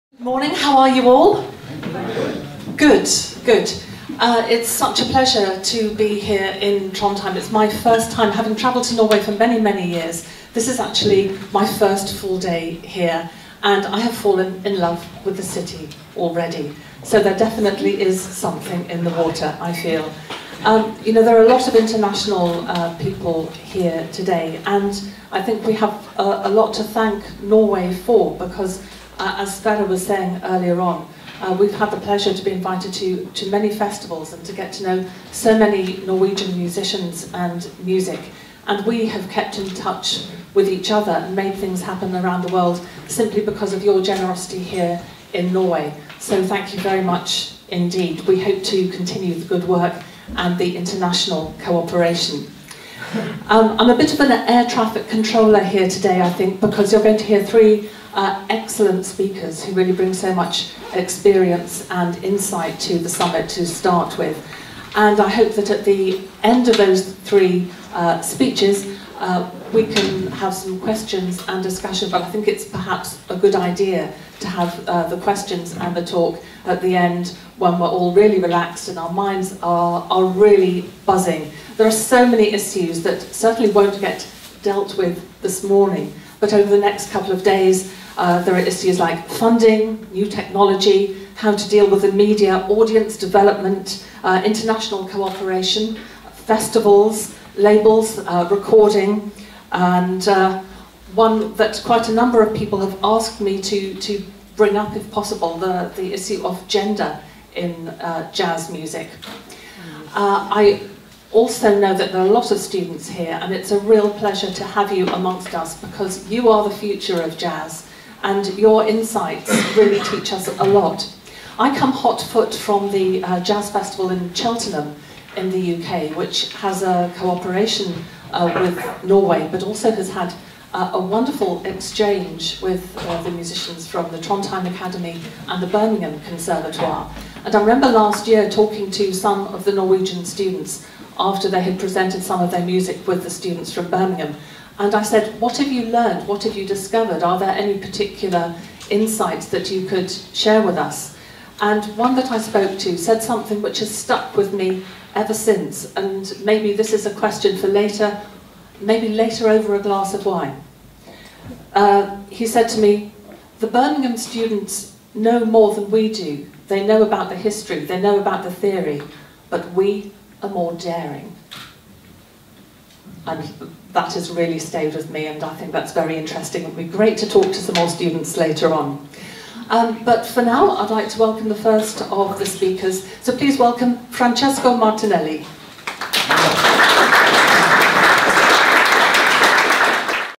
The Jazz Summit 2012- 3: Fiona Talkington
Fiona Talkington of BBC Radio

Jazzfest in Trondheim, Norway, invites you to take a critical look at the state of jazz, jazz education, and where the road goes from here. Get a glimpse of the new generation of Norwegian jazz, and find out what they "give them for lunch in Trondheim".

Trondheim Jazz Festival takes place medio May annually.